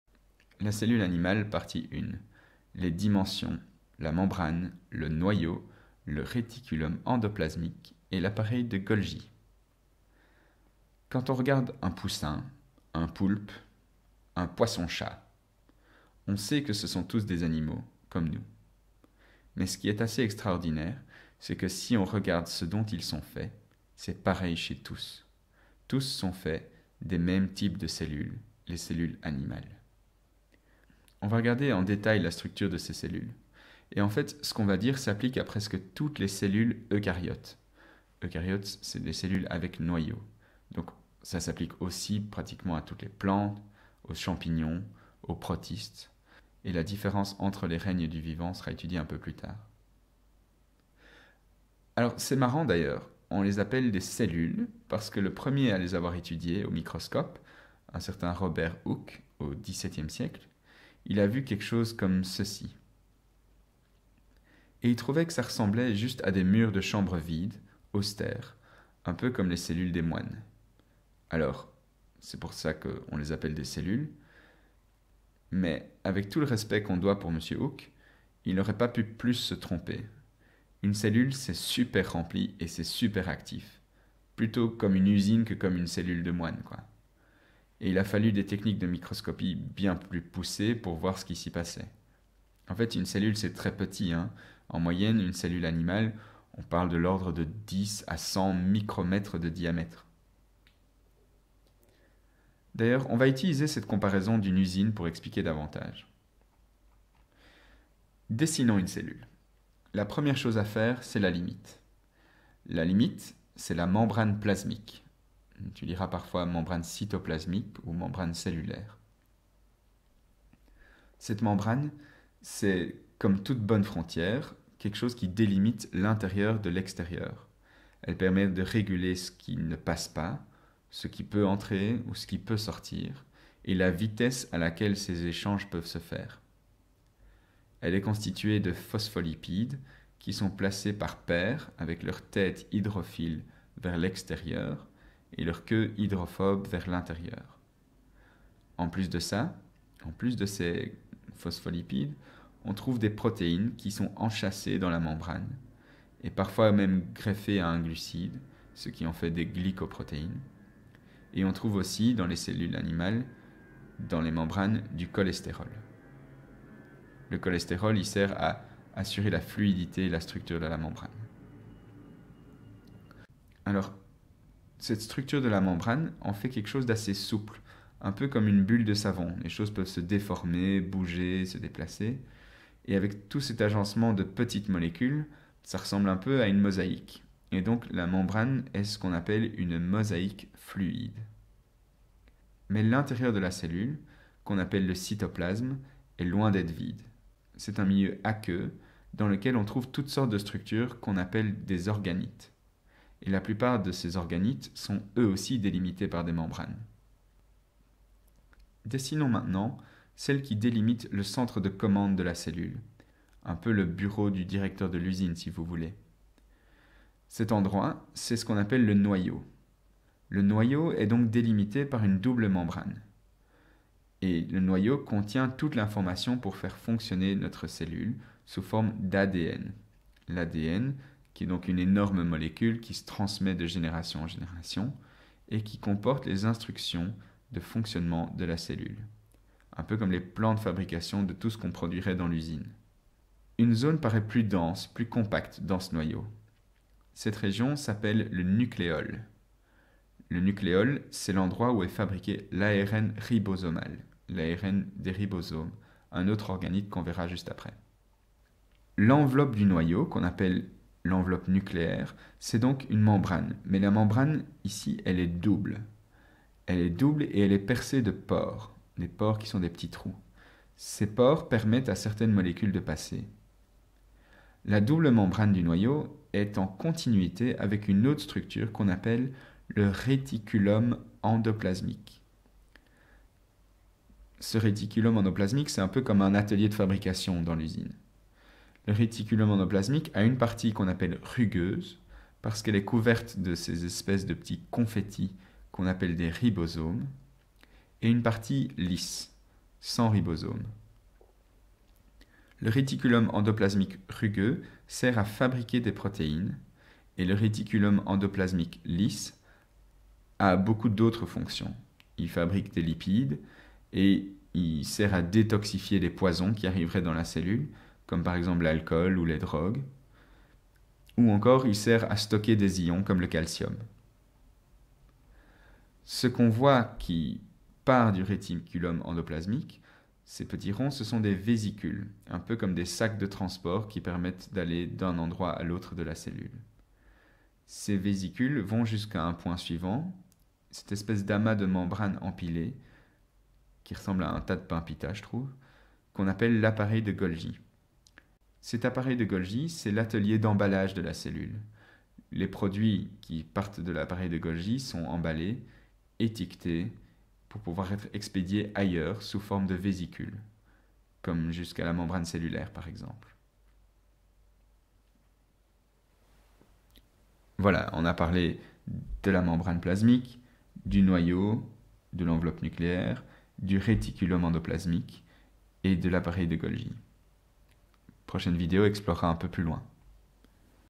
Structure cellulaire 1 - Membrane, noyau, appareil de Golgi
Cette vidéo explore la première partie de la structure cellulaire. Le lien vers le quiz sur cette vidéo